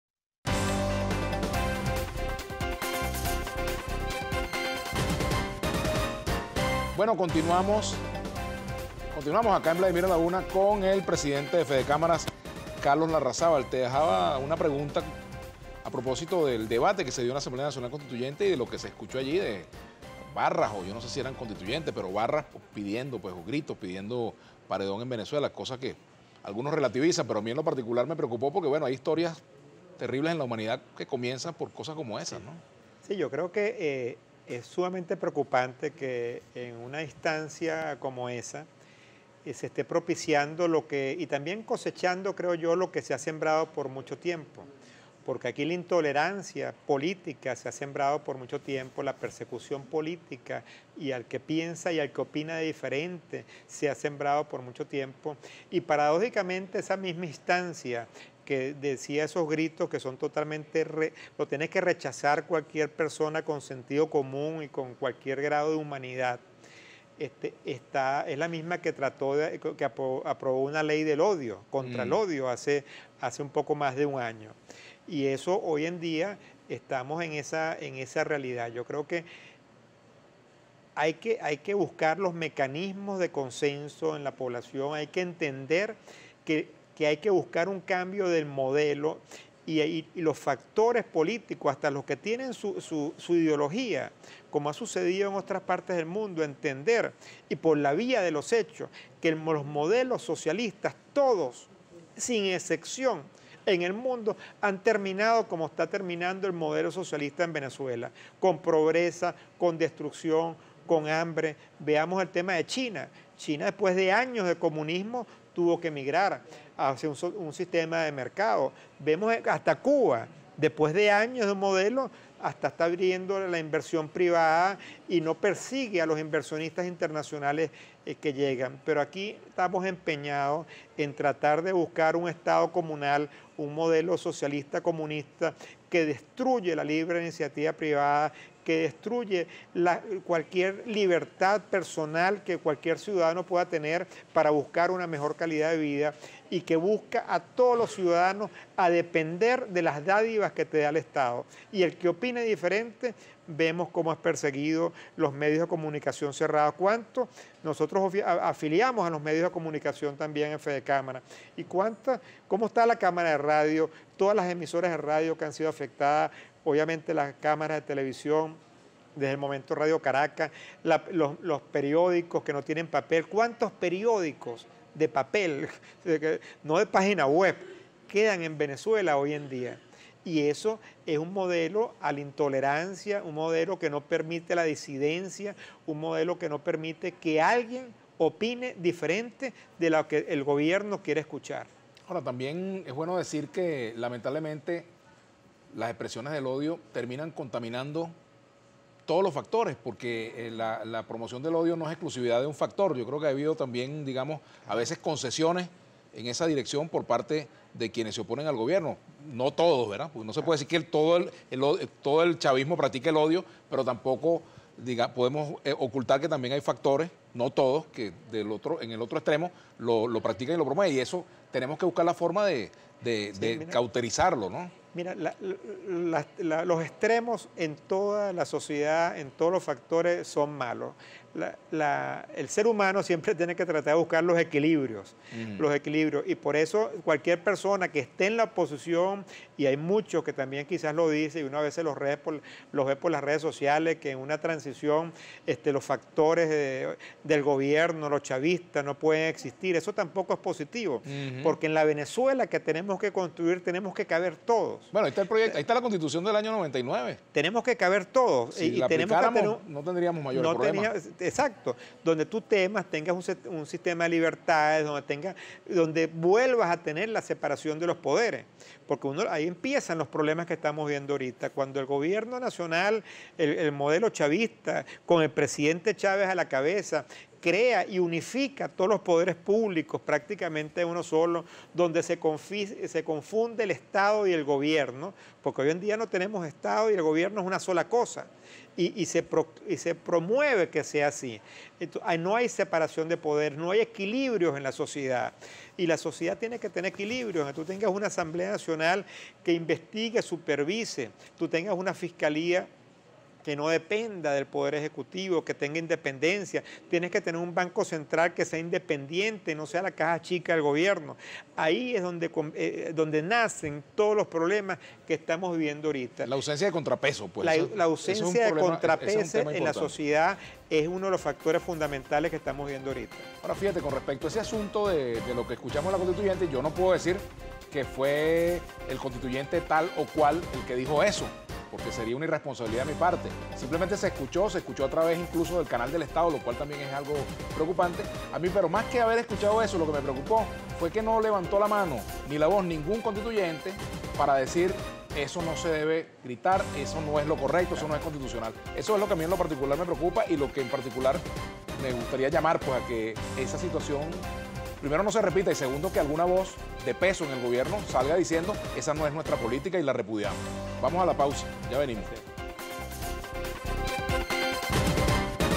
Fedecámaras: "Se reportan en el Zulia pérdidas por alrededor de los 800 millones de dólares" 4:5
El presidente de la institución, Carlos Larrazábal aseguró que la productividad del país se ha visto afectada tras los apagones. Advirtió que la situación en el Zulia, específicamente, ha sido la más dramática y alertó que "los canales de distribución y logística han sido destruidos, abastecer a Maracaibo será más problemático", dijo.